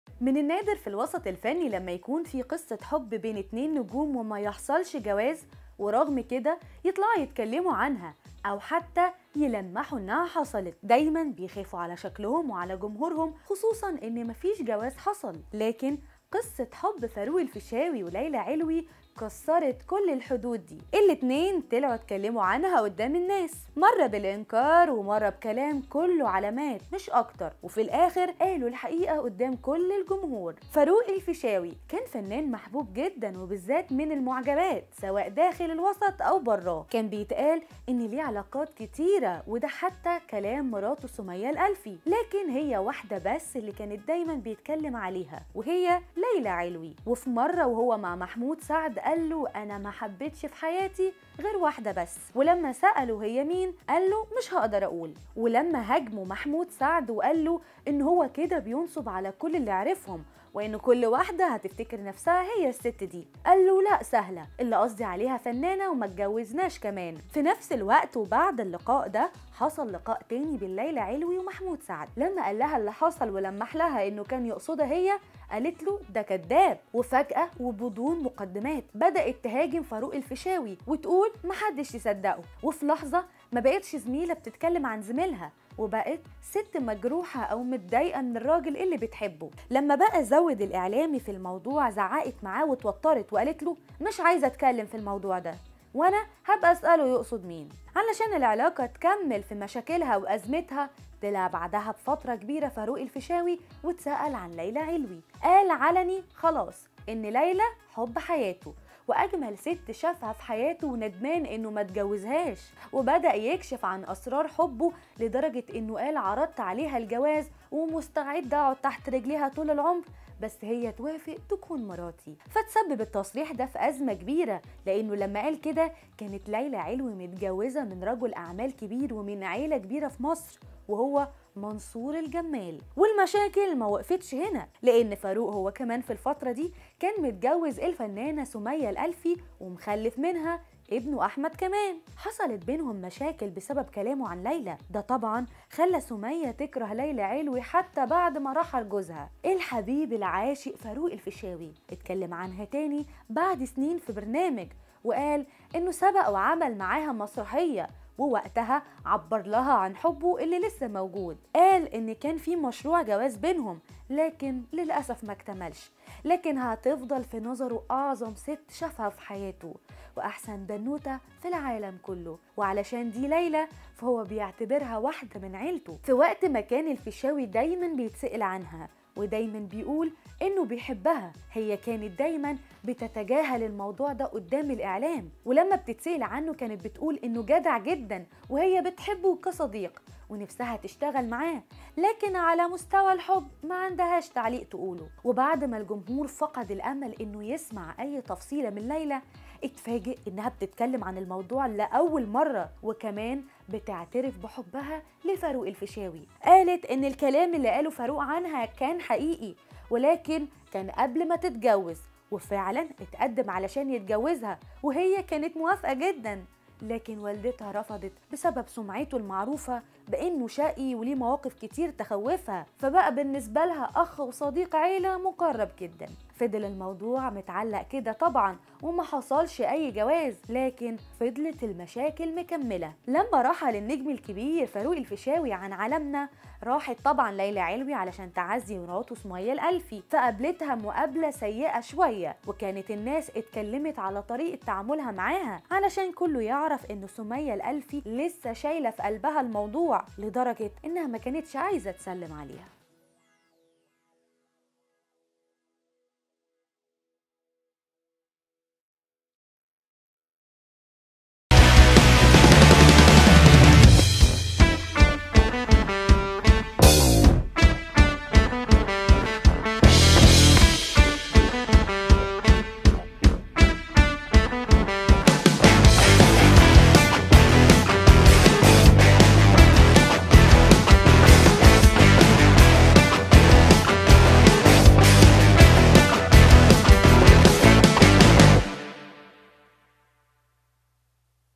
مستعد أقعد تحت رجليها بس هي توافق!أجمل فنانة مصرية أحبها فاروق الفيشاوي واتسببلها في مشاكل مع جوزها؟
من النادر في الوسط الفني لما يكون في قصة حب بين إتنين نجوم ومايحصلش جواز ورغم كده يطلعوا يتكلموا عنها ,, أو حتى يلمحوا إنها حصلت
دايمًا بيخافوا على شكلهم وعلى جمهورهم خصوصًا إن مفيش جواز حصل
لكن قصة حب فاروق الفيشاوي وليلى علوي كسرت كل الحدود دي